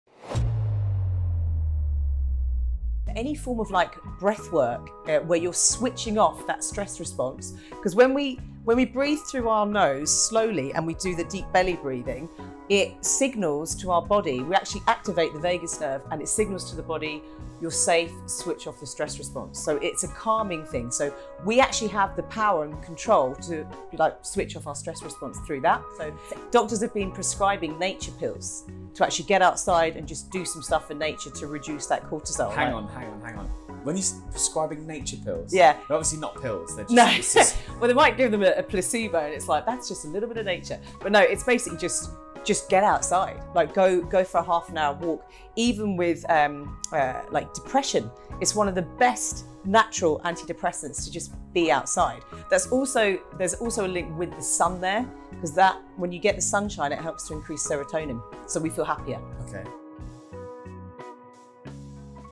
Why doctors have been prescribing nature pills!
This week we get the chance to dig deep into the mind of NLP coach Victoria Tipper and here her tips on how we can maximize our full potential!

Follow us on The Recipe to Success Podcast with Daniel Leese platforms for the full audio and video versions!